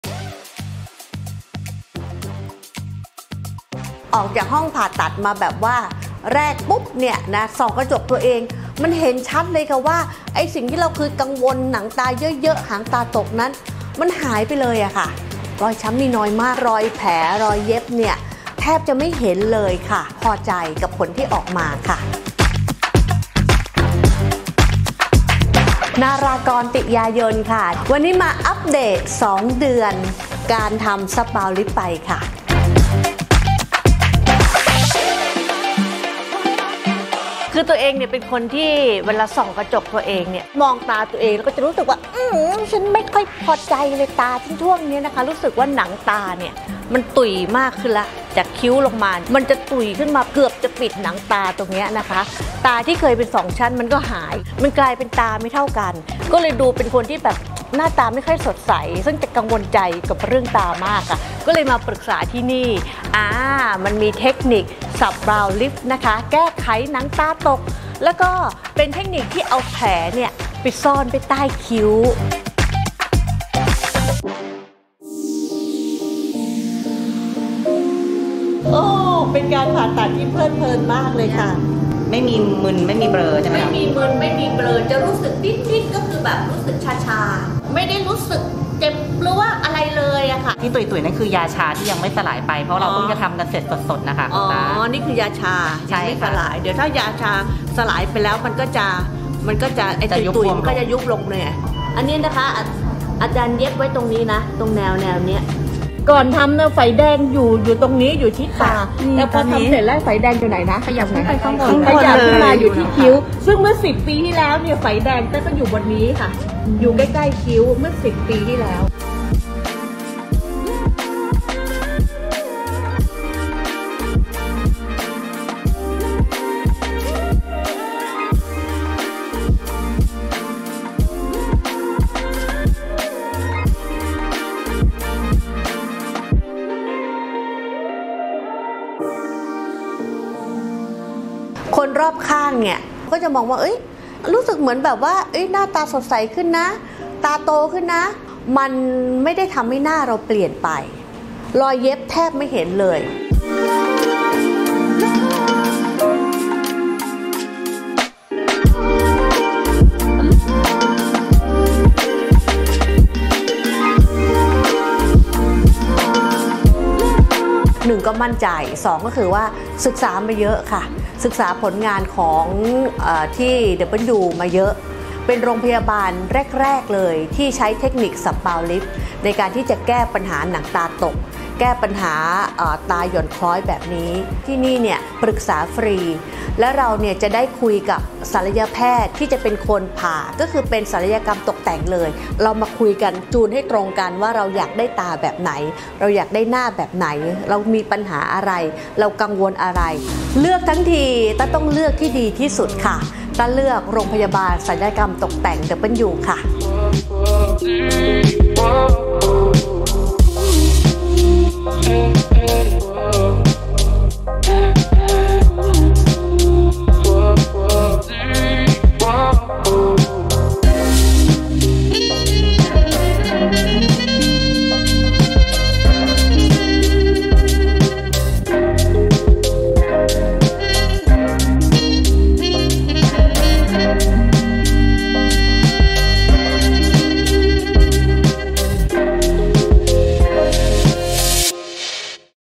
คุณต๊ะ นารากร‼ แก้ไขปัญหาหนังตาตก ด้วยเทคนิคซ่อนแผลไว้ใต้คิ้ว Sub Brow Lift
คุณต๊ะ นารากร‼ แก้ไขปัญหาหนังตาตก ด้วยเทคนิคซ่อนแผลไว้ใต้คิ้ว Sub Brow Lift “ชั้นตาชัด รอยแผลแทบมองไม่เห็น ชอบมากค่ะ” ความประทับใจหลังทำกับศัลยแพทย์เฉพาะทางที่โรงพยาบาลศัลยกรรมตกแต่งดับเบิ้ลยู

ราคาเริ่มต้น 49,000 บาท ‼ ปกติ 60,000 บาท

🔹 แก้ปัญหาหนังตาตก ชั้นตาไม่ชัด
🔹 ซ่อนแผลไว้ใต้คิ้ว  บวมช้ำน้อย พักฟื้นระยะสั้น
🔹 แก้ปัญหาเฉพาะบุคคลโดยแพทย์เฉพาะทาง

🏢 มาตรฐานโรงพยาบาล ดำเนินการโดยแพทย์เฉพาะทาง